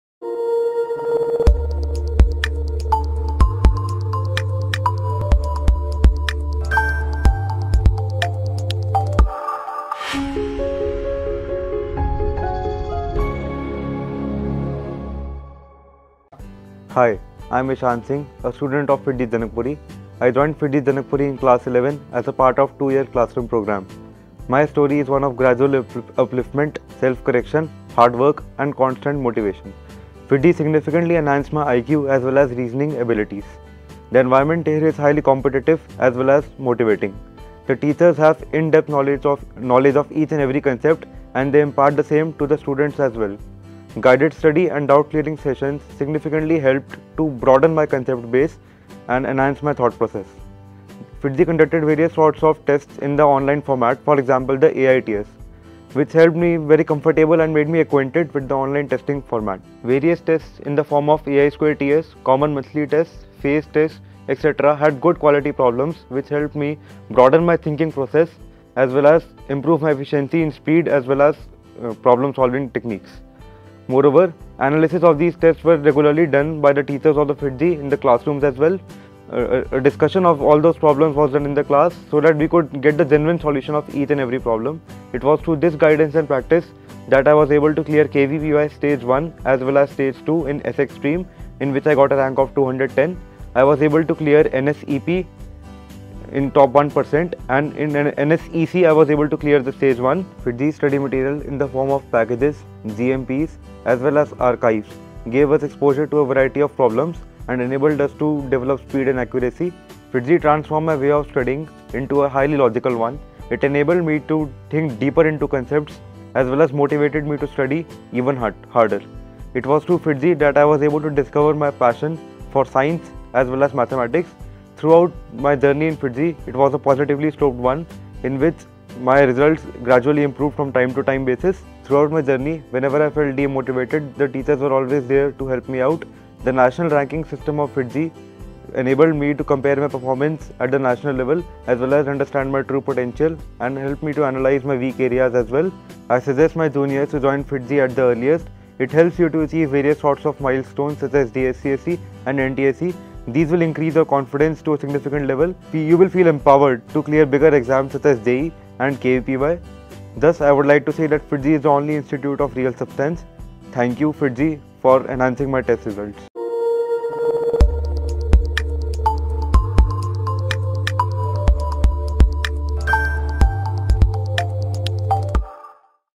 ISHAAN SINGH All India Rank 59 JEE ADVANCE, 2019
Heartiest Congratulations to our Student ISHAAN SINGH from FIITJEE (Janakpuri) Centre on securing All India Rank 59 in JEE ADVANCE, 2019!🏆 Let's hear from ISHAAN what he wants to say about his success...